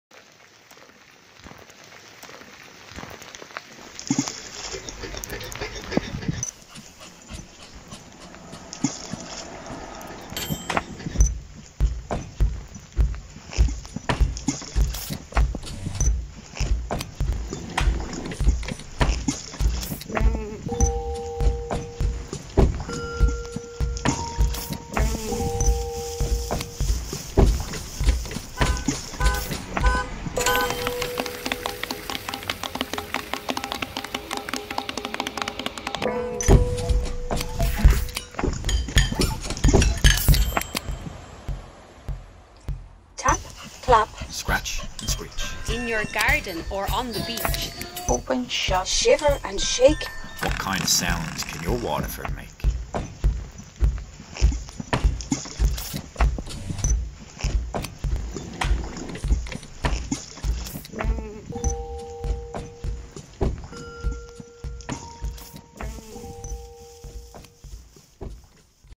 Sounds like Waterford - send us your sounds!
"Tap, clap, scratch and screech,
in your garden or on the beach.
Open, shut, shiver & shake,
what kinds of sounds does your Waterford make?"

We want you to collect the sounds of your Waterford!
Find an interesting sound, make a short video and send it to us.
All the found sounds submitted will be made into the Well Festival Sounds Like Waterford virtual rhythmic creation!

Submit your sounds to us by WhatsApp to (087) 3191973 until Sunday 18 October.

Follow us in Instagram:
music generation waterford